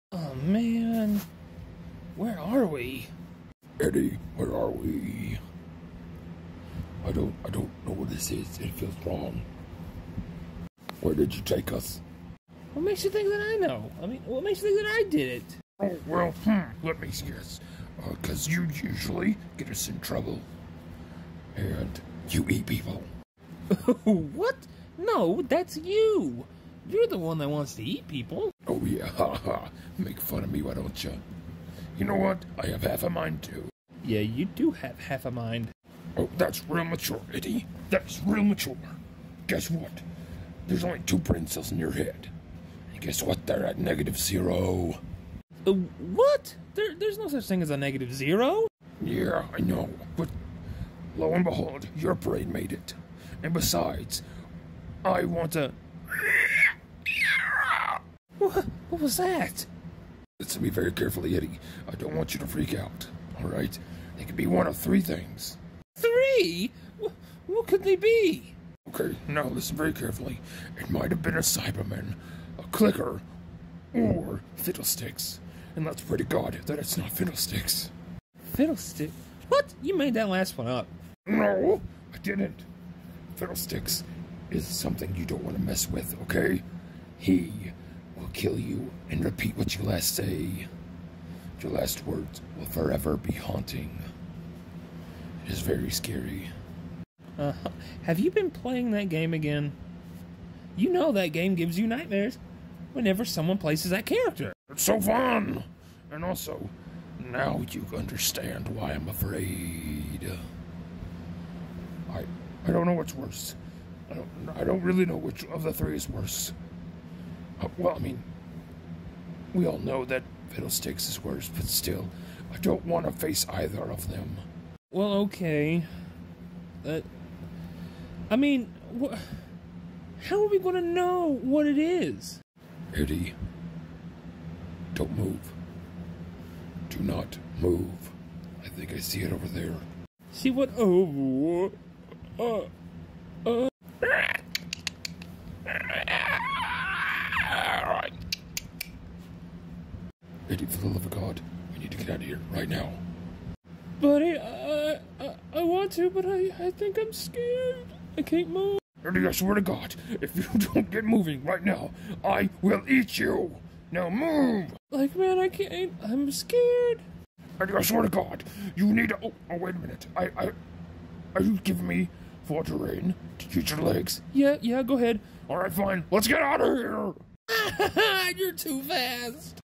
Venom and Eddie in a random place
Venom and Eddie are in a new place. Where could they be?greenscreen